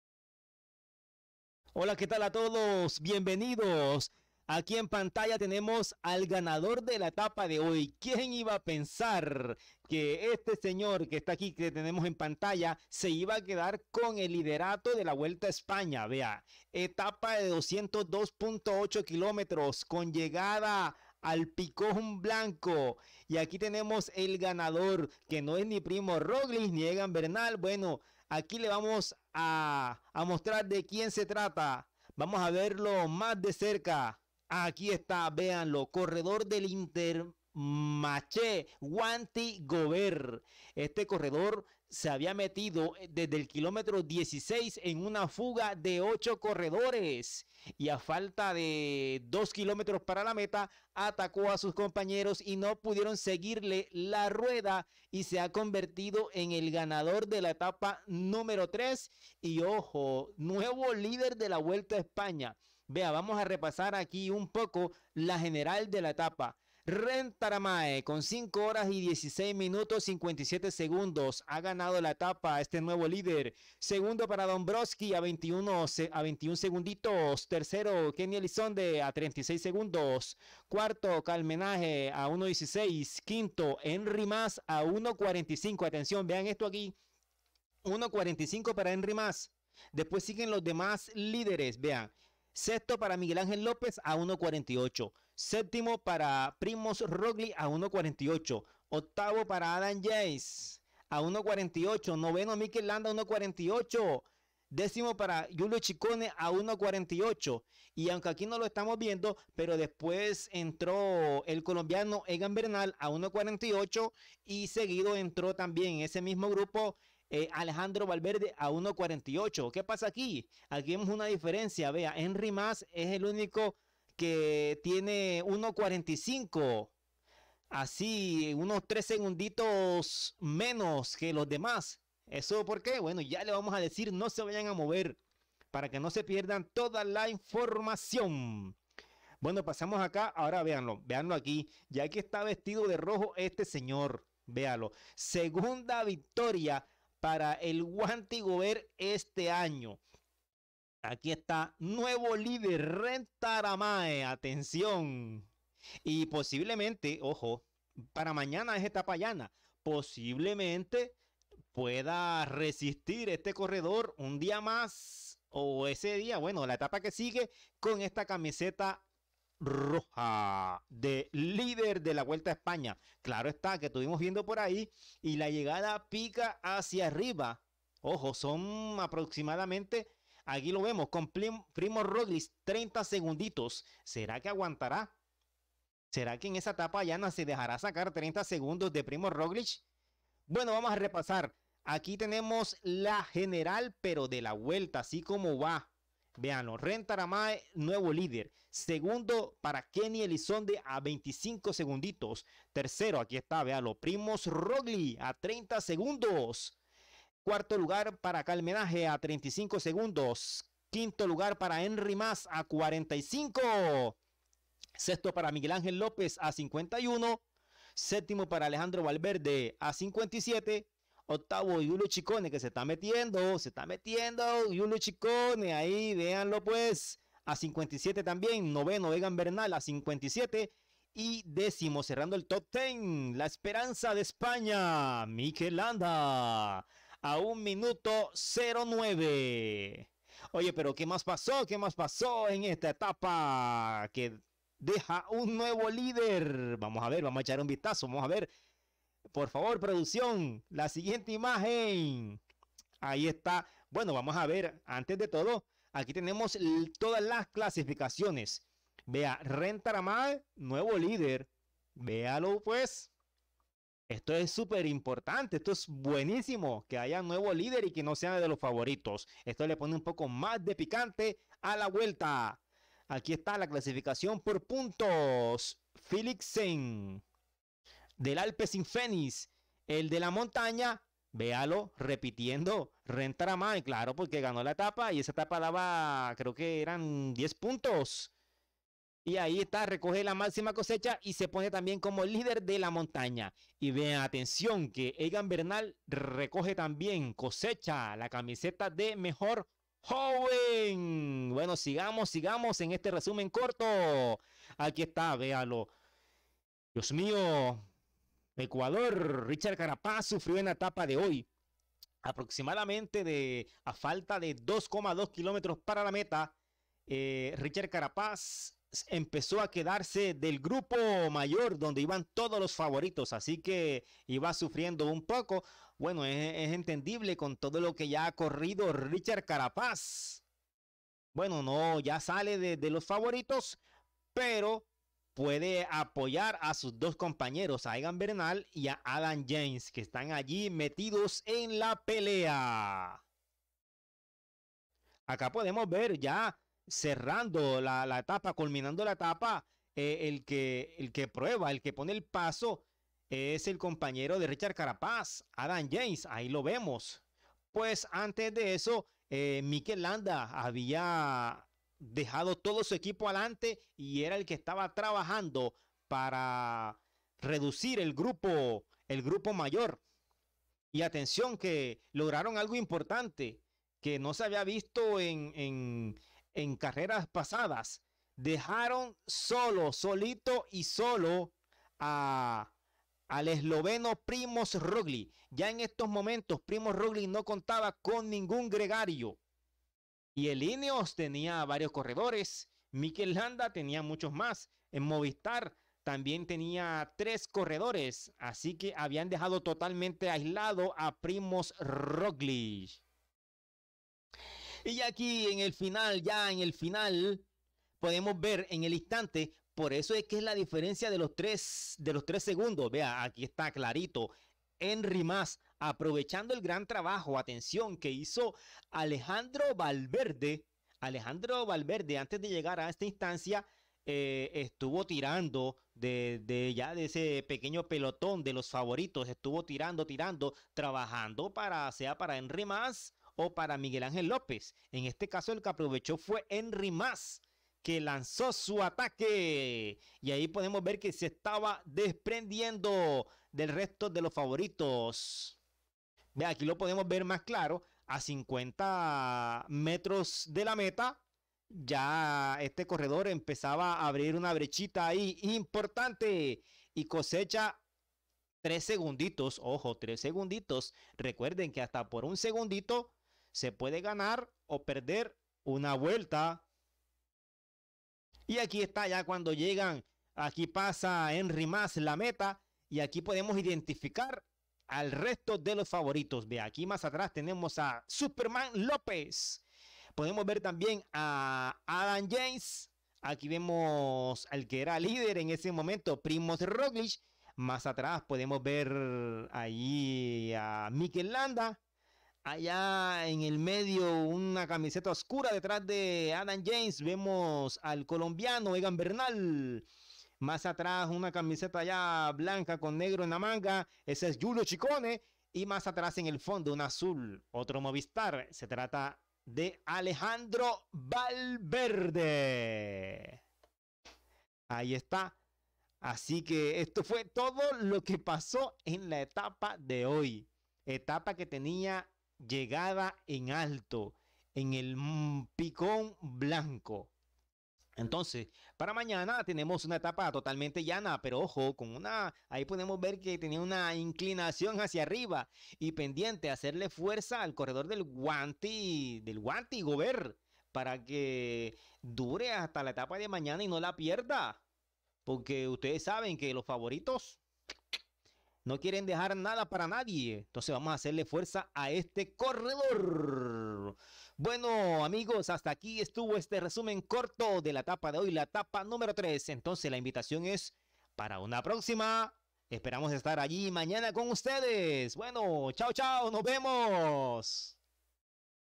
VUELTA ESPAÑA 2021 . MOVISTAR ATACA . NUEVO LIDER . LA FUGA GANA HOY
SE MOVIO LA CARRERA HOY EN EL PICON BLANCO DONDE GANA UN CICLISTA DE LA FUGA Y SE METE COMO NUEVO LIDER DE LA VUELTA . CARAPAZ SUFRE...